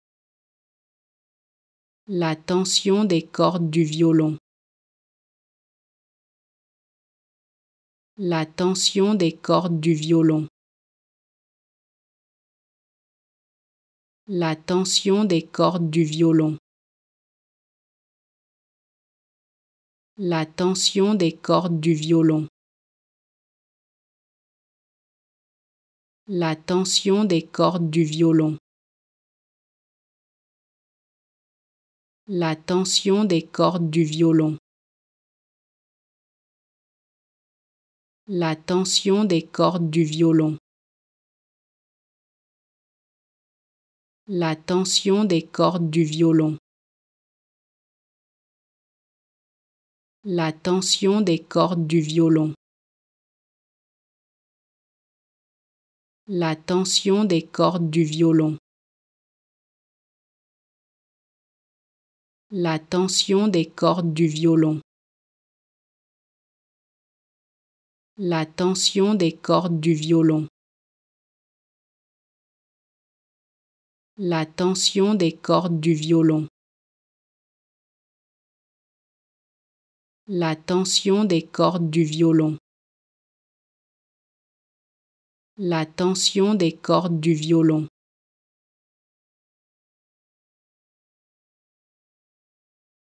How to Pronounce ''La tension des cordes du violon'' Correctly in French
Learn how to say and properly pronounce ''La tension des cordes du violon'' in French with this free pronunciation tutorial.